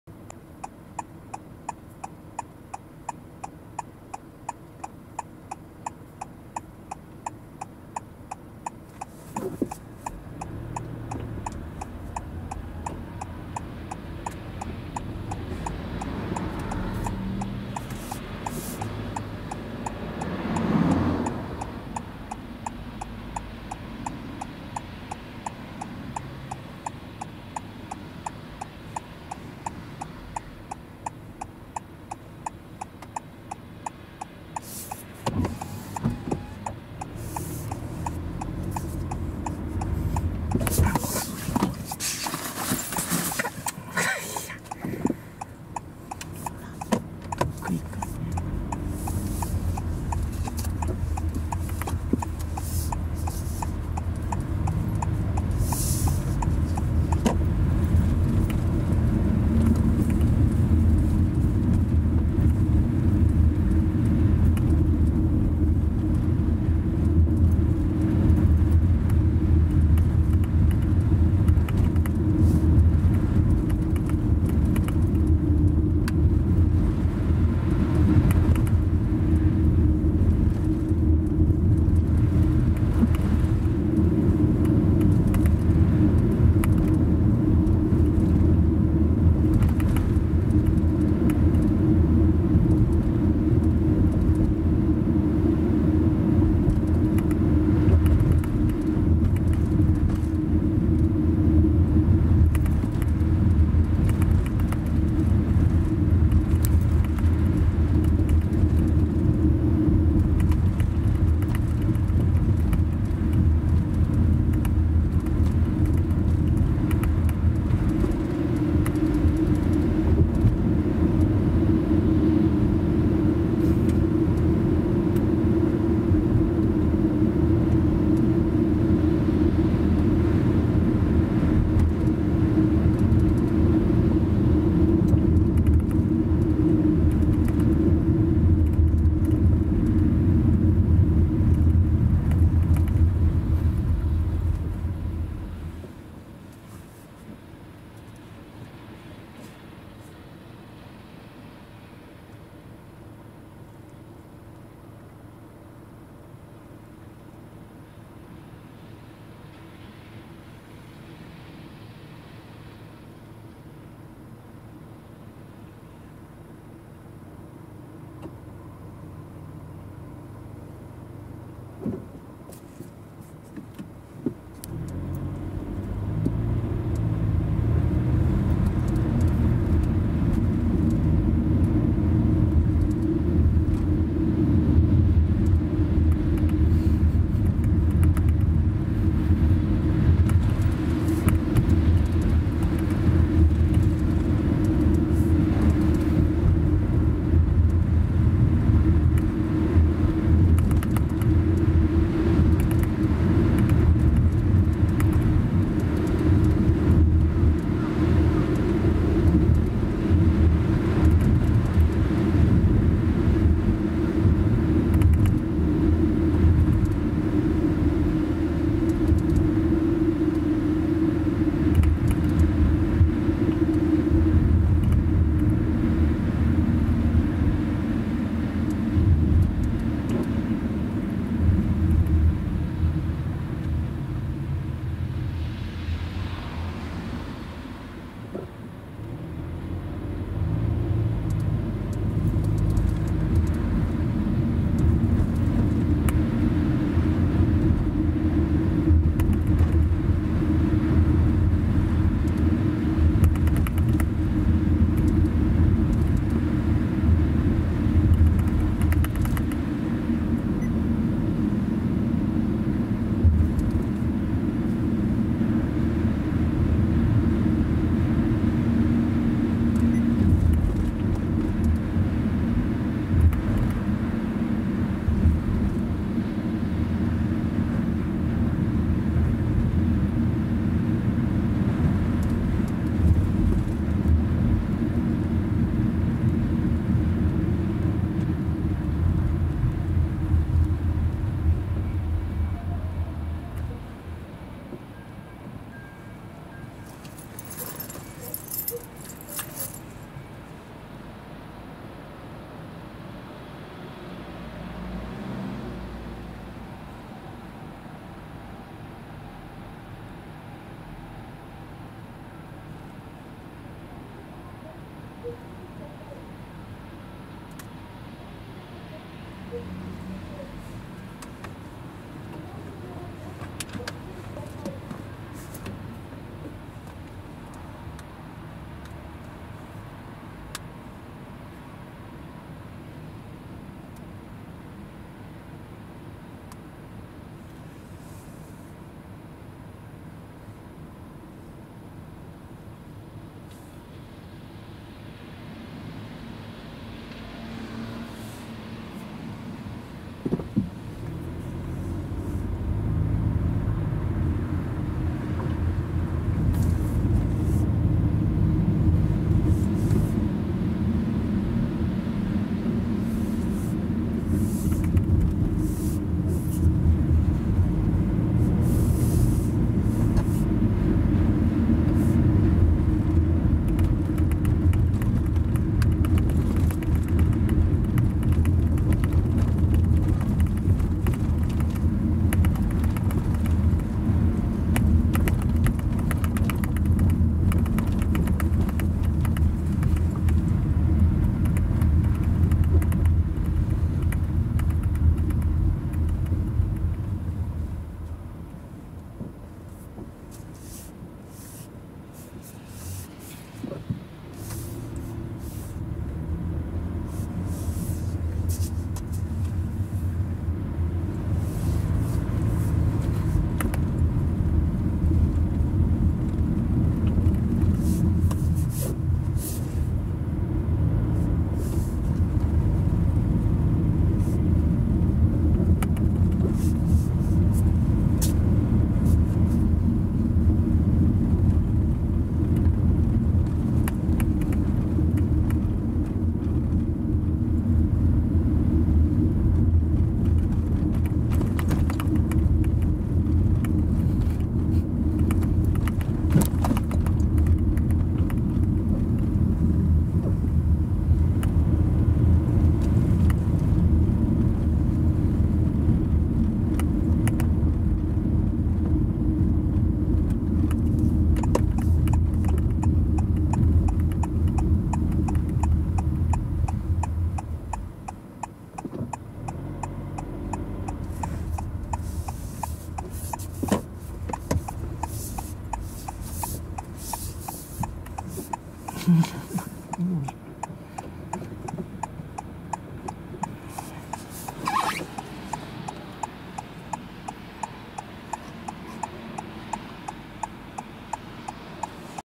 西8丁目北線 → 北33条西線[ 札幌市 4K/30p 車載動画 雪道ドライブ]：北20条中通交点～琴似栄町通交点
西8丁目北線、北33条西線をドライブしました。ドライブ
距離は、2.4km、時期：2021年1月下旬。

0:00 北20条中通・新琴似連絡線交点
2:20-2:50 北26条線交点
3:05 北27条線交点
4:15 西8丁目北線から北33条西線
4:50-5:50 国道5号/札幌新道交点
6:30 新川横1号線交点
7:40 琴似栄町通交点（以上です）